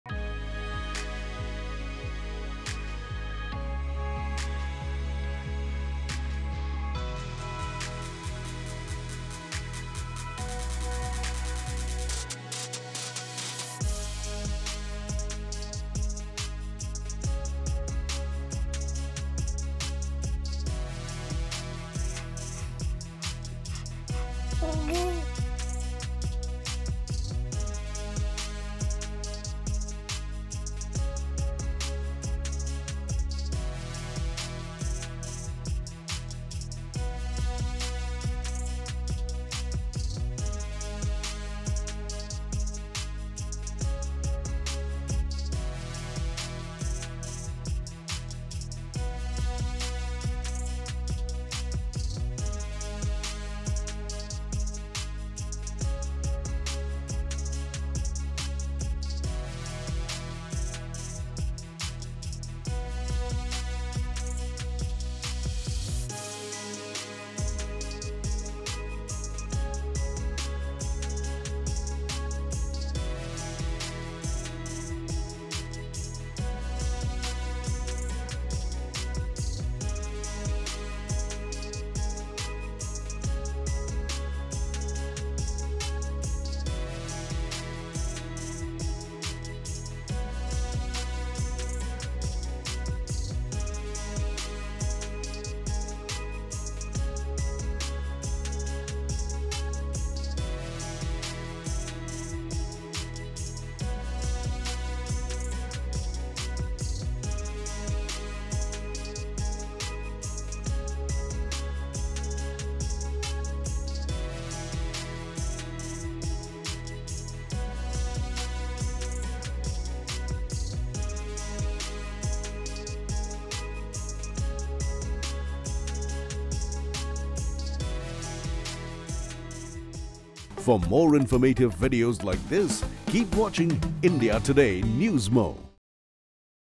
Your Child's 'Goo-Goo-Ga-Ga' Is Not Nonsense Babble, They Actually Talk
What if we tell you that your baby talks to you. As per new research, these cute, seemingly nonsense sounds might actually be the babies warming themselves up for speech. Watch to know all about it.